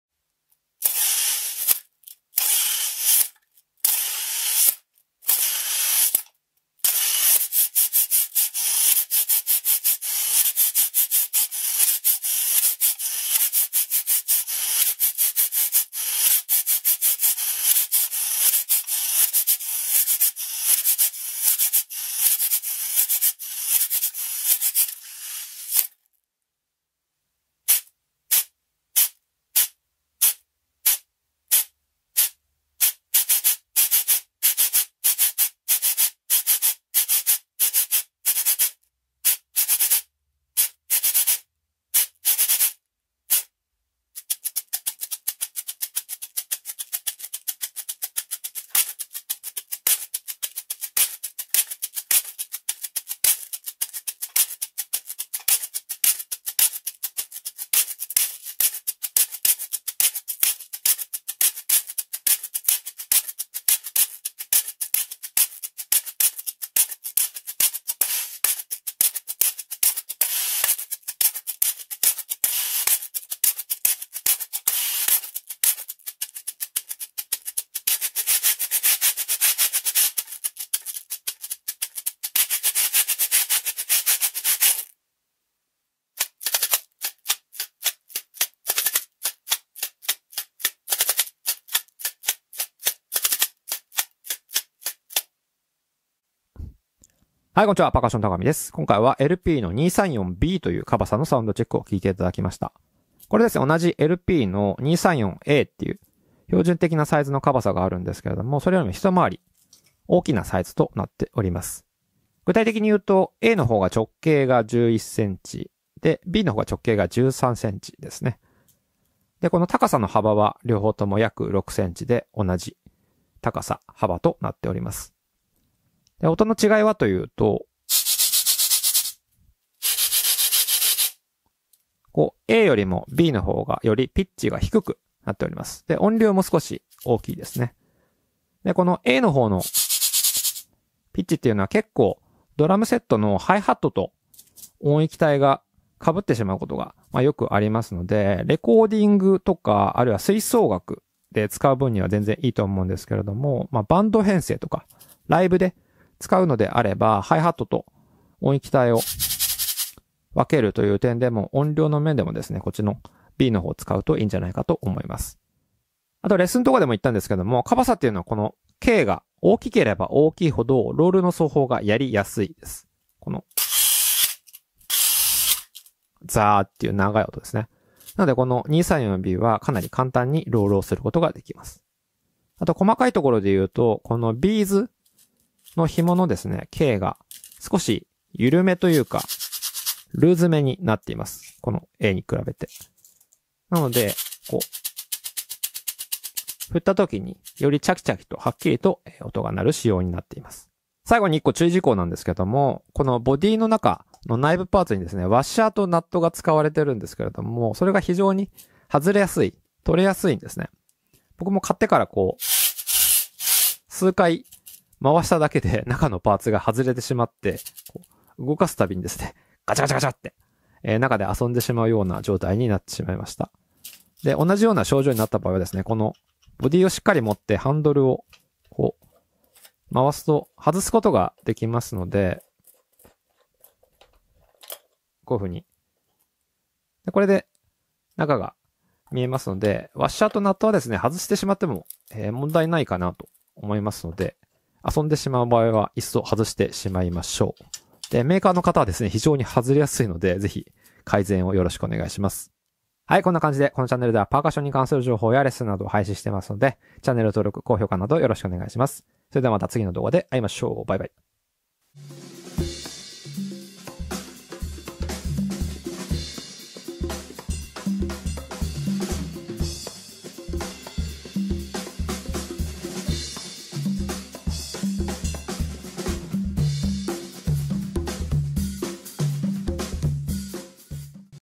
【カバサ】LP234B Afuche-Cabasaのサウンドチェック＆レヴュー
LP234Bのサウンドチェック＆レヴューです、楽器選びの参考にして下さい。比較に出しているのはLP234Aです。

使用マイク AKG C214